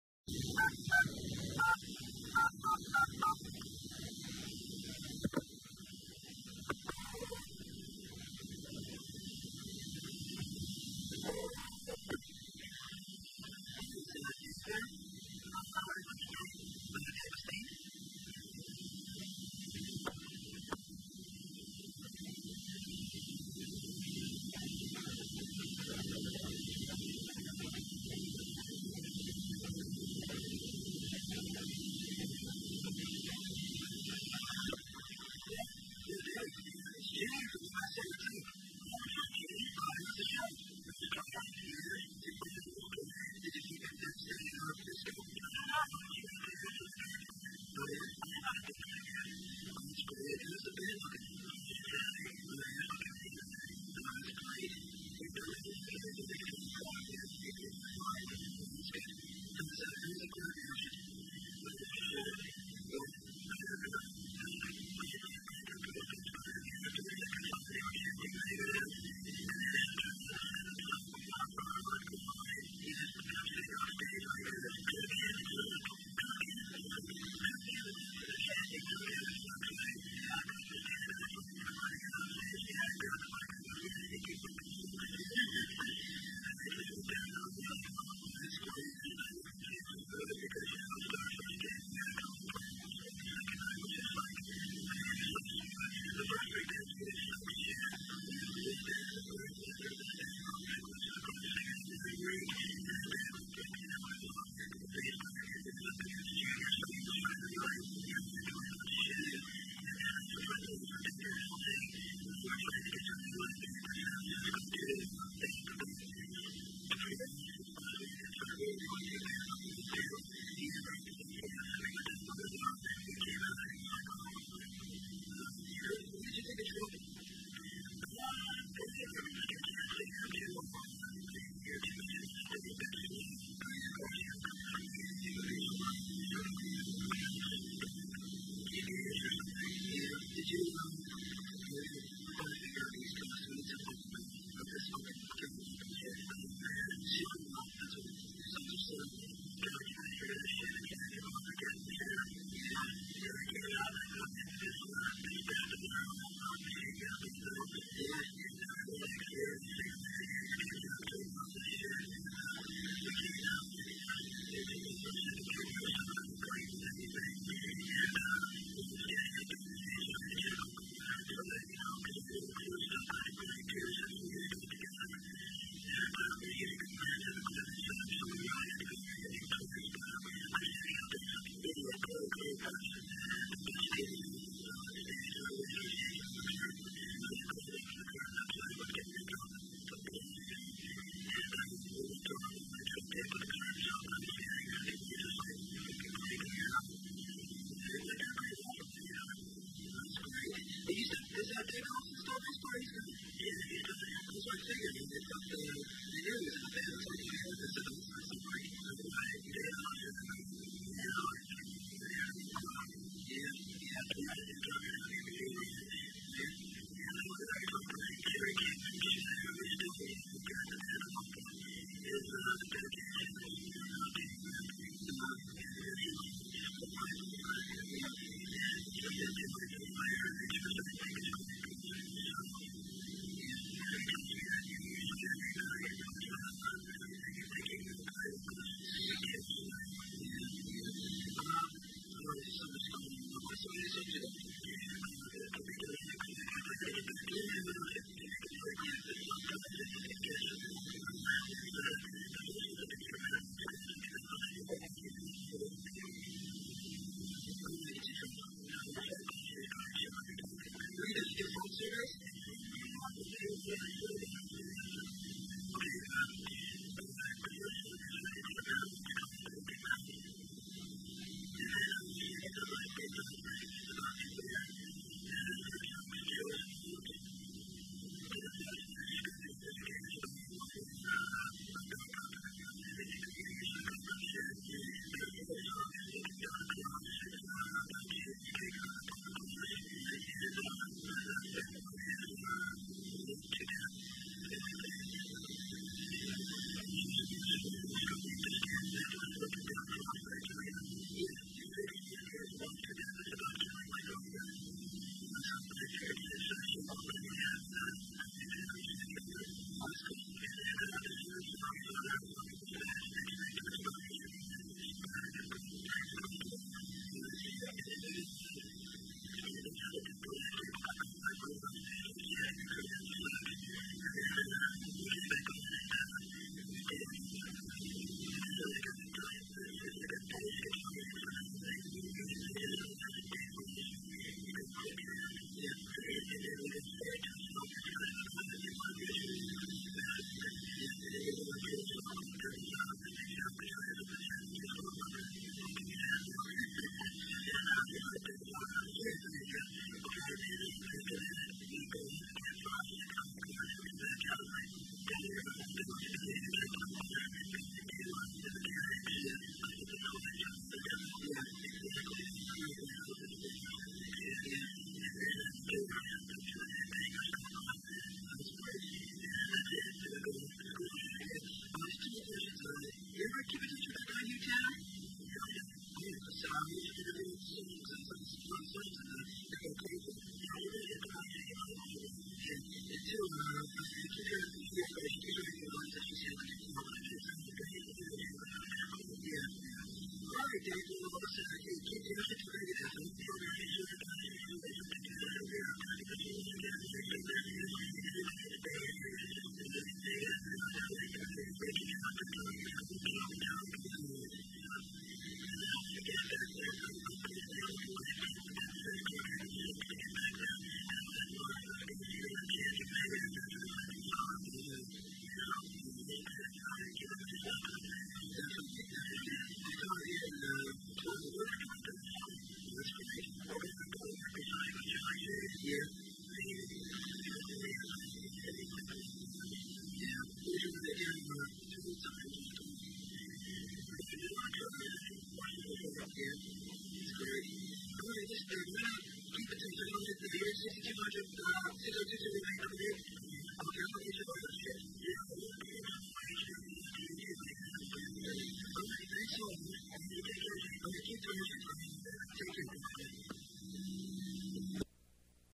Rare Megadeth Interview 1984.
This is from a 1984 bootleg, phone itnerview while Dave is at work, very cool interview, this is before they were even signed and only after they played a couple of shows.